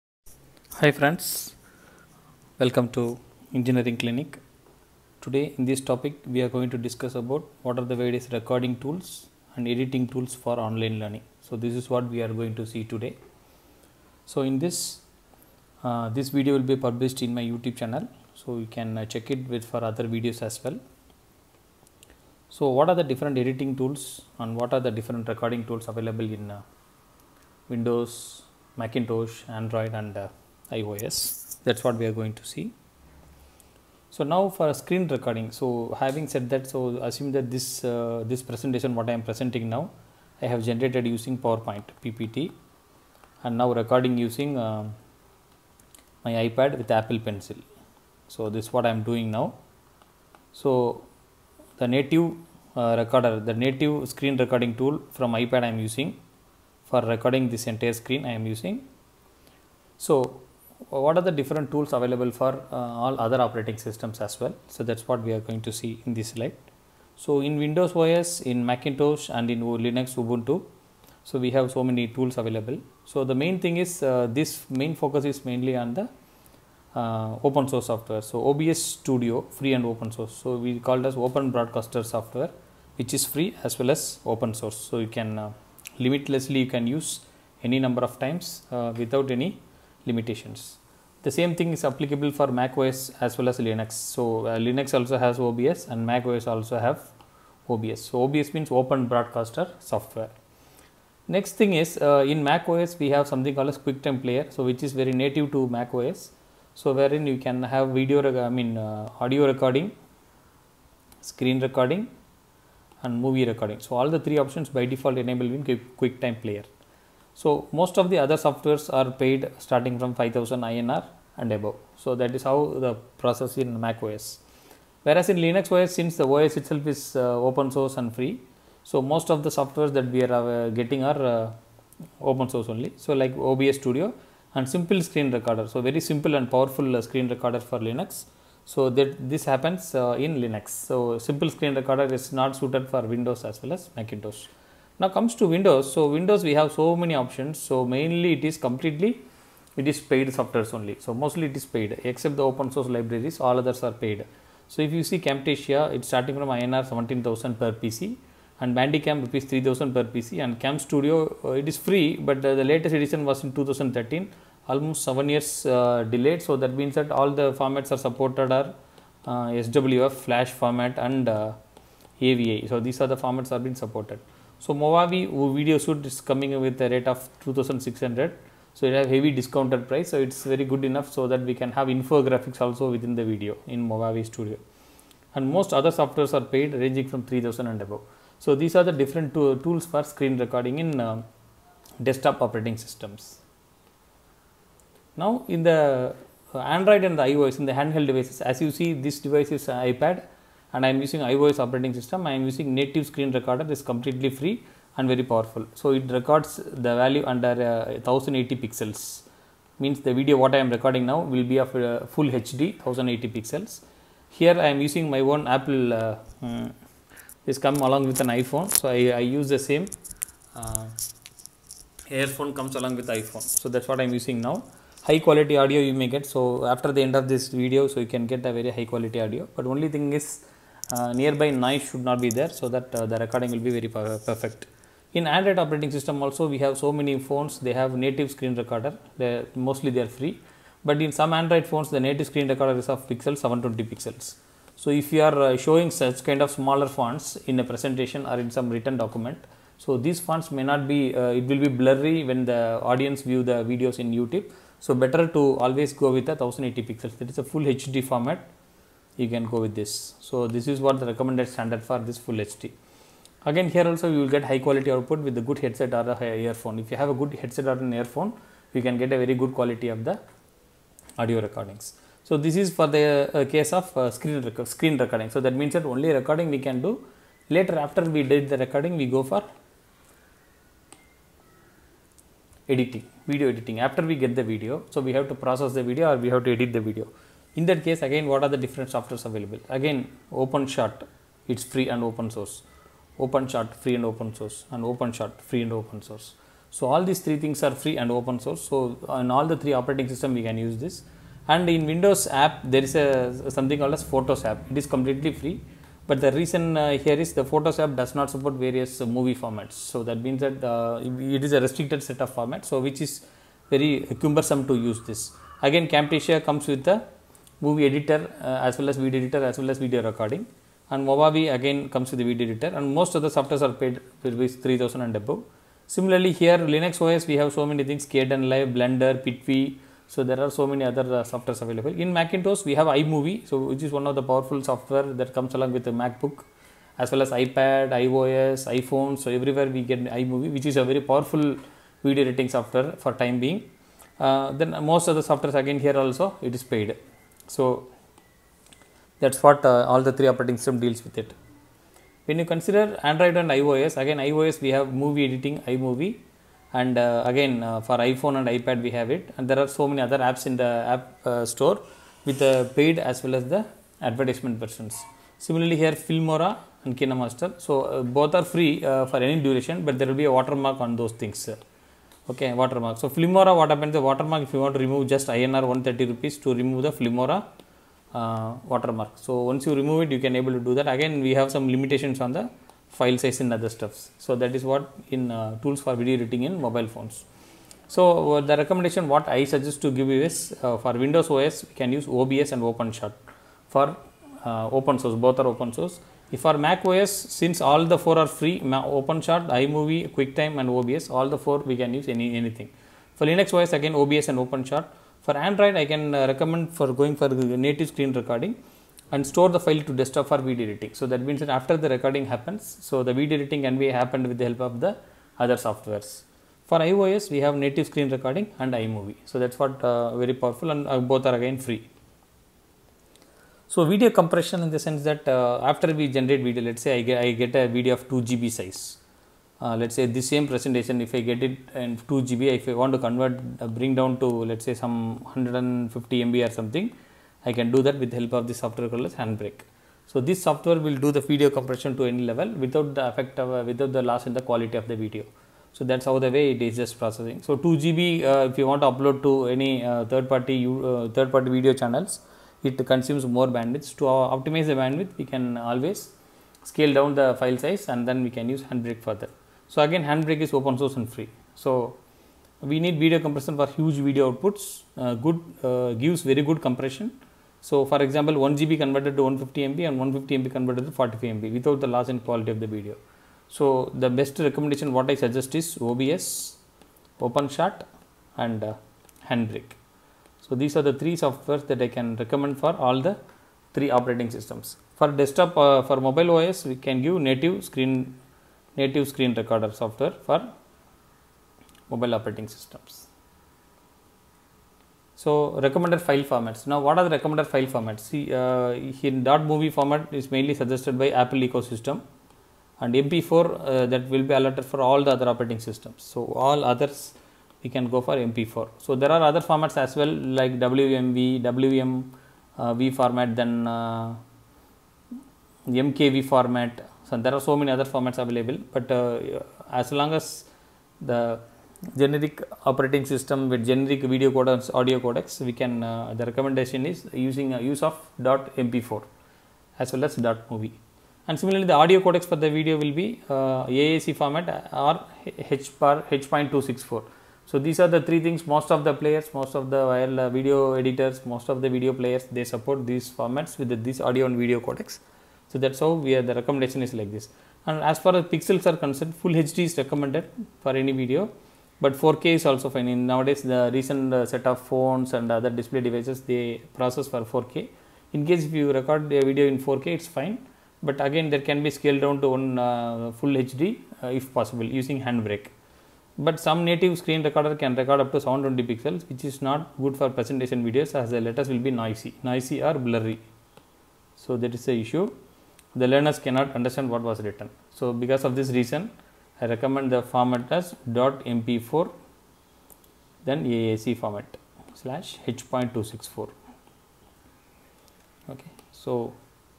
Recording and video editing tools for Online Learning
This short video showcases the different tools and techniques for Screen Recording and Video Editing tools for Online Learning.